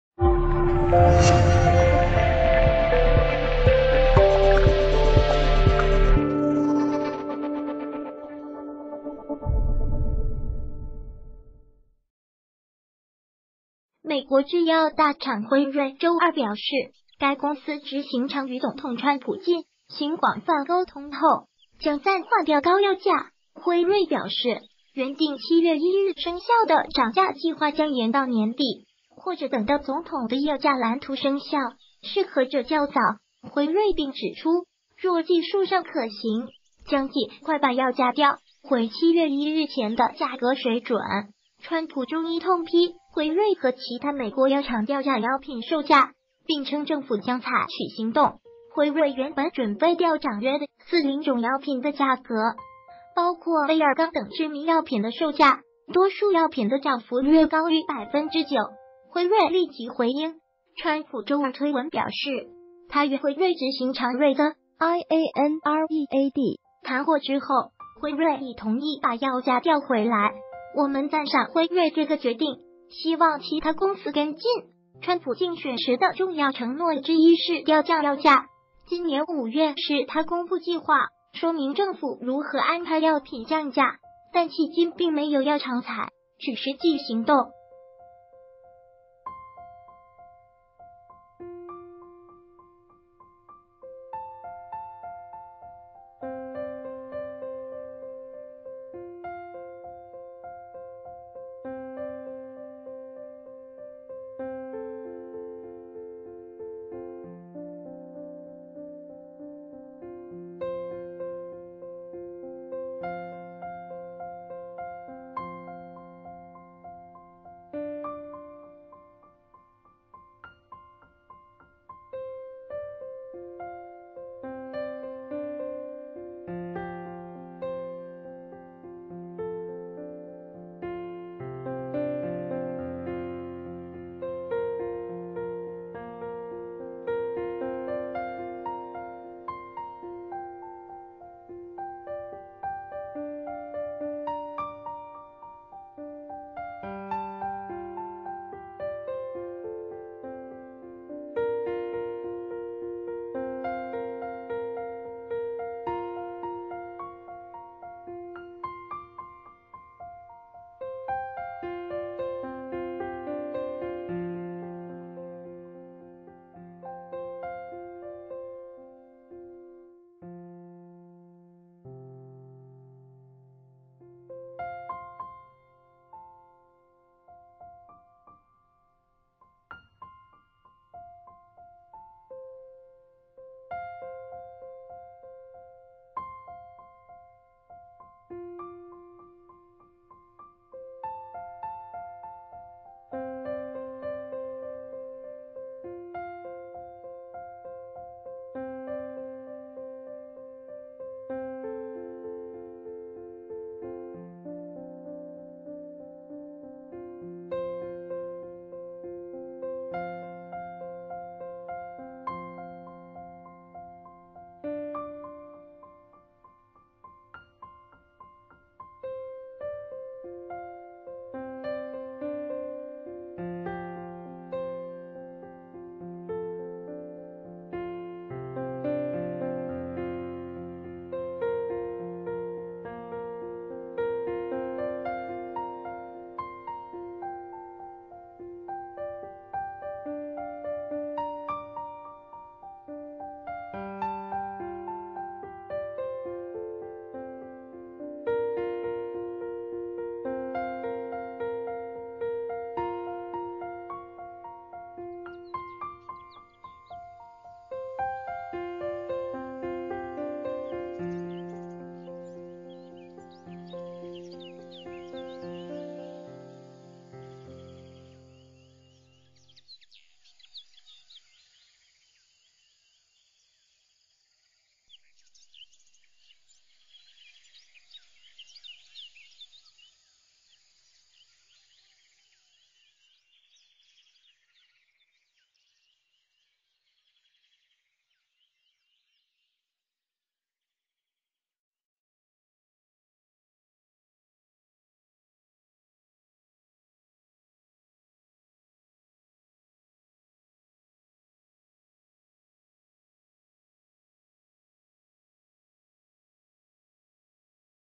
川普狂轟葯價 美製葯大廠輝瑞暫時不漲價了
谢谢！川普狂轟葯價 美製葯大廠輝瑞暫時不漲價了